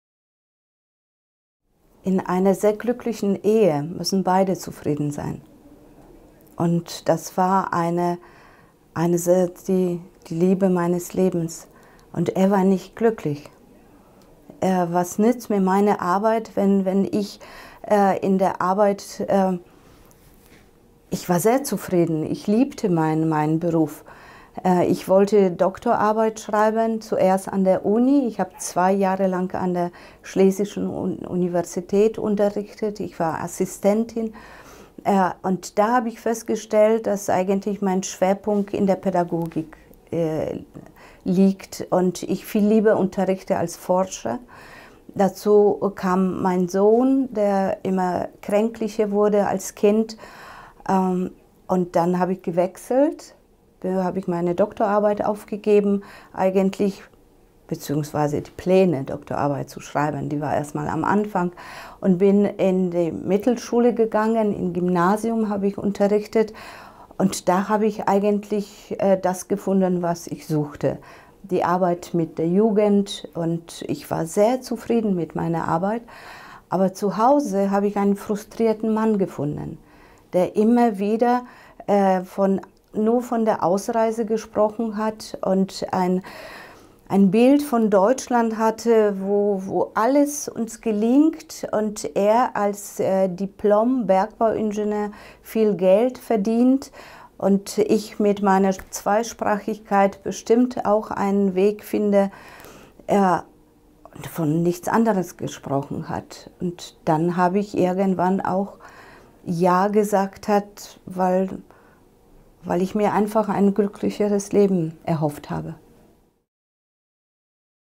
Barbara Surma: Hoffnung auf ein glückliches Leben
Barbara Surma wuchs als Kind deutschstämmiger Eltern in Polen auf. Mit ihrem Mann siedelte sie in den 80er Jahren nach Deutschland über, obwohl sie ihr Leben in Polen mochte. Im Video nennt sie die Gründe für ihren Weggang.